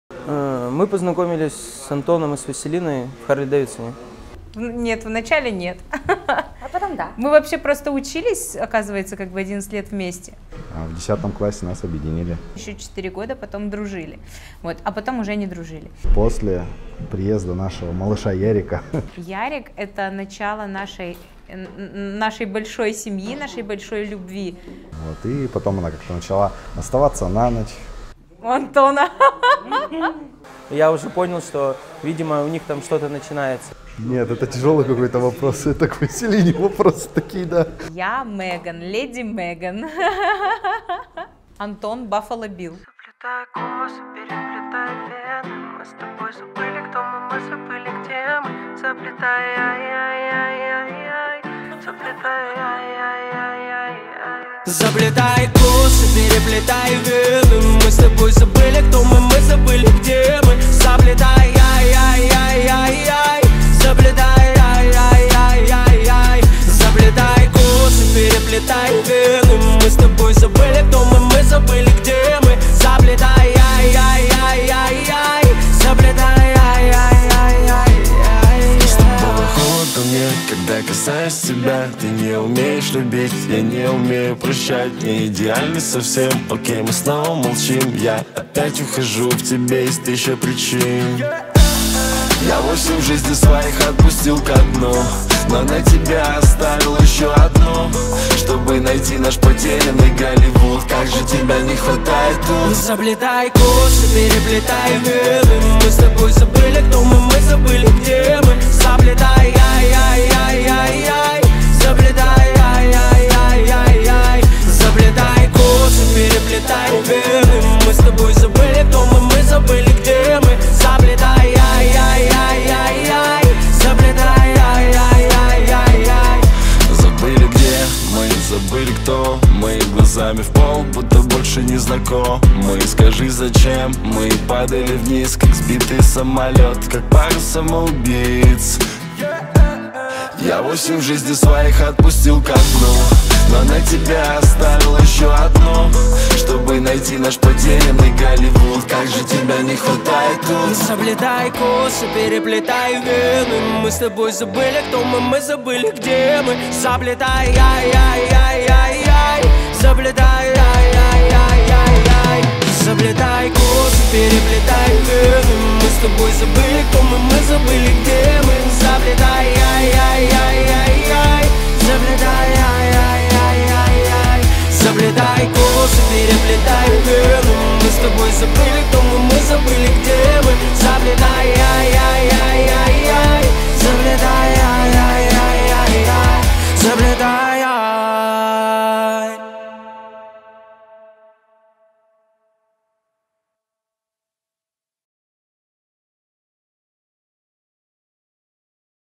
SDE - Свадебное видео - Хабаровск
Антон и вероника - снято и смонтировано в один день, для показа гостям в конце вечера